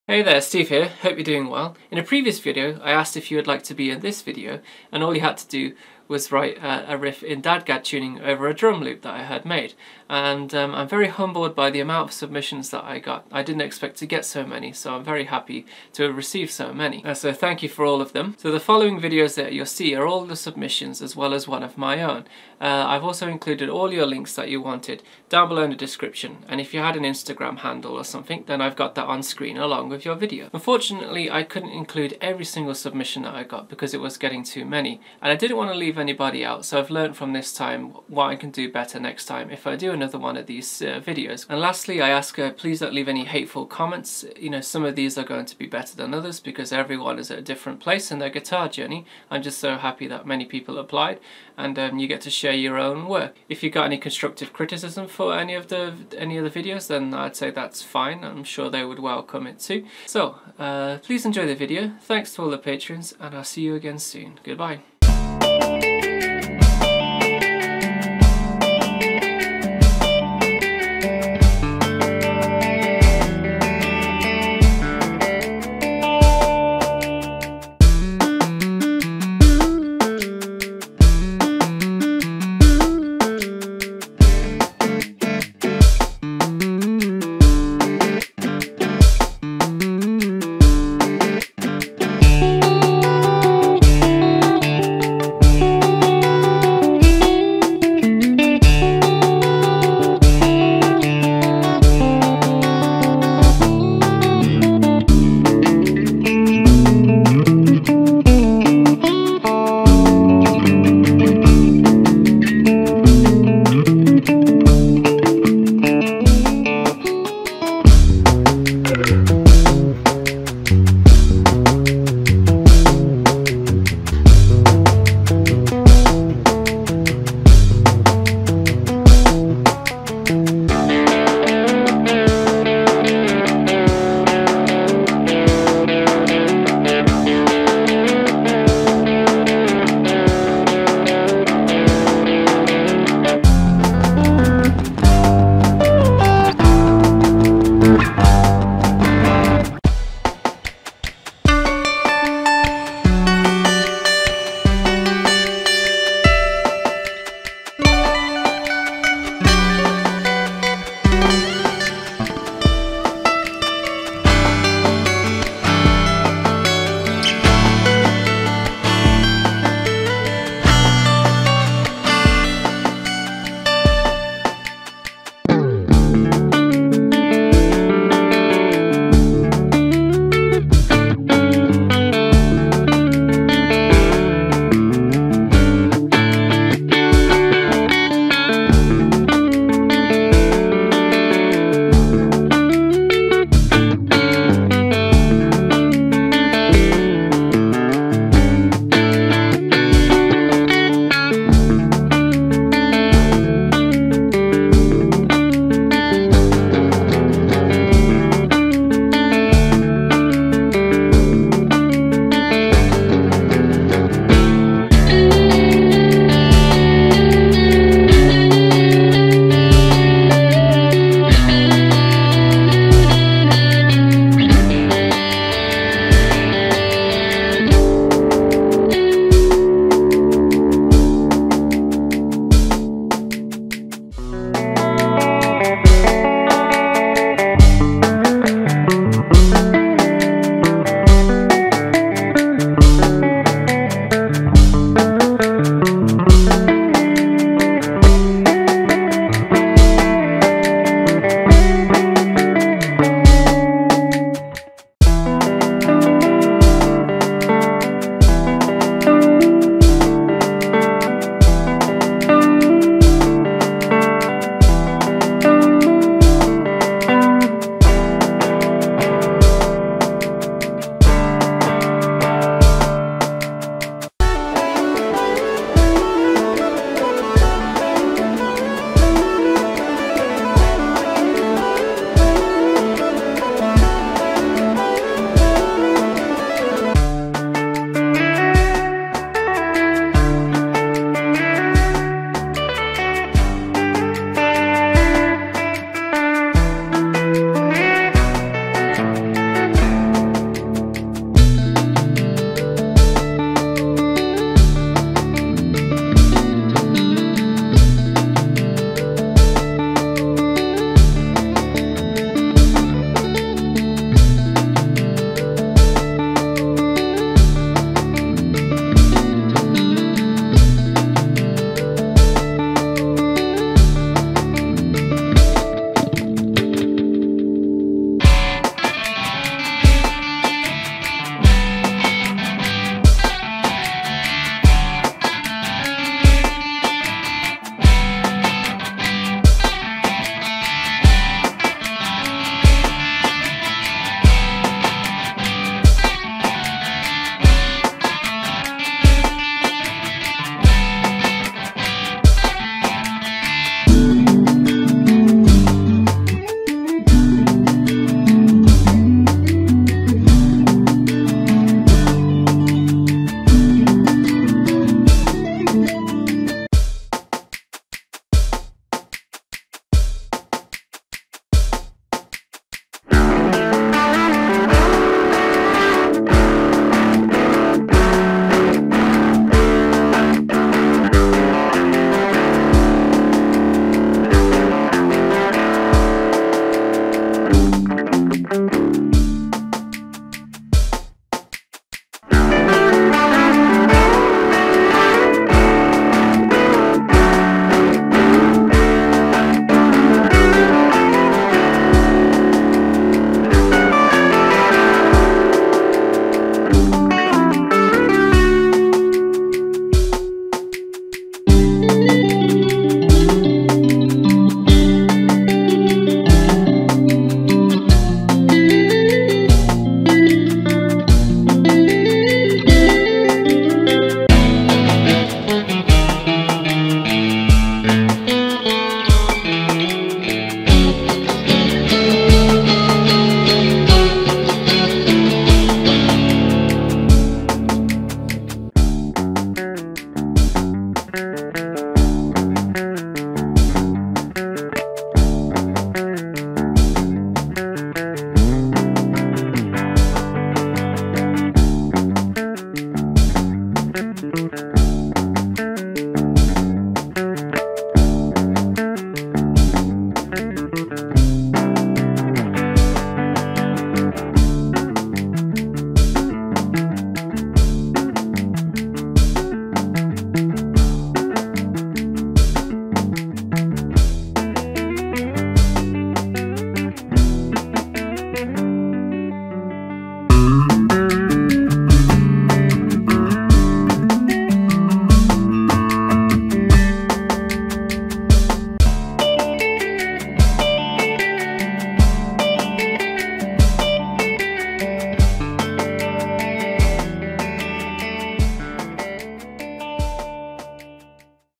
Beautiful DADGAD Math Rock Riffs - Super Tuning Friends
Super Tuning Friends Episode 3 - DADGAD Guitar Tuning
The challenge was to write a riff in DADGAD tuning over a drum loop I made.
Thanks so much to everyone who took the time to write and submit something, and sorry to those who didn't make the final cut! Let me know if you would like to see and/or participate in another tuning wars video.